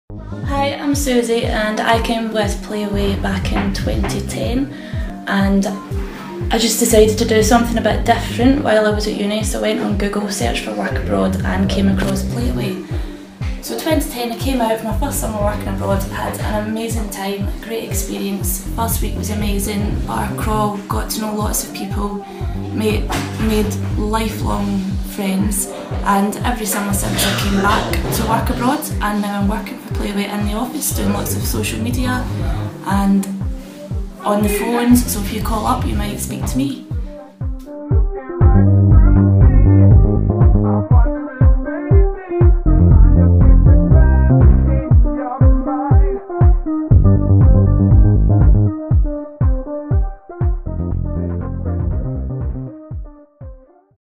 PlayaWay Abroad Review - Meet Suzie
Suzie from Glasgow came away with PlayaWay a few years ago and now works in the office, here's her short review about her experience.